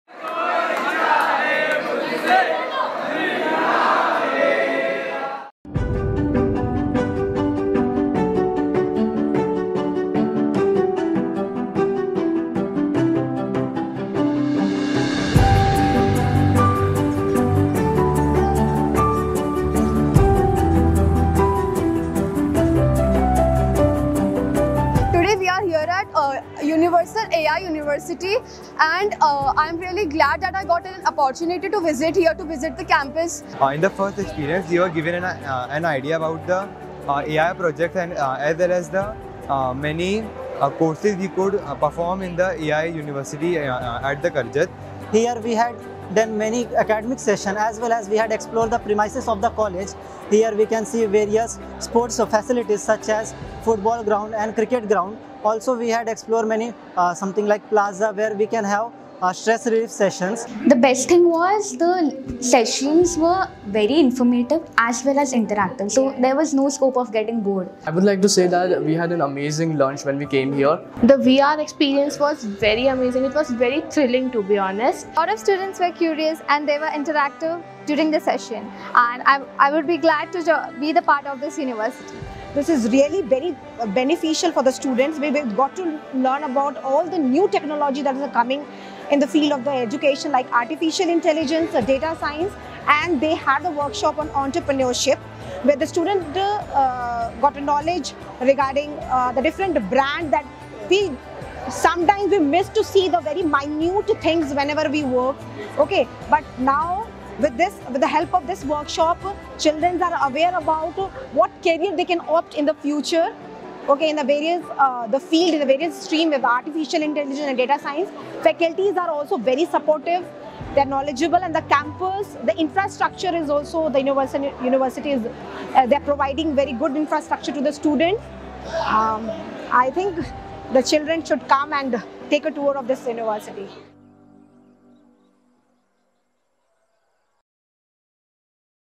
Empowering Minds: Sadhu Vaswani School Student Explore the Future at Universal Ai University, Karjat
Join us on an exciting journey as students from Sadhu Vaswani International School, Navi Mumbai, share their transformative experience at Universal Ai University, Karjat. On January 16th, 2024, these young minds delved into the realms of technology, global business management, design programs, psychology, and economics.

The hands-on workshops took them through the cutting-edge AI Labs, Quantum Computing Labs, the Global Cross Asset Trading Centre, and Virtual Reality, providing an immersive and insightful experience.

Witness their exploration of the future, the impact of technology on various disciplines, and their takeaway from this enriching educational journey.

UNDERGRADUATE PROGRAMS
AI & Future Technologies School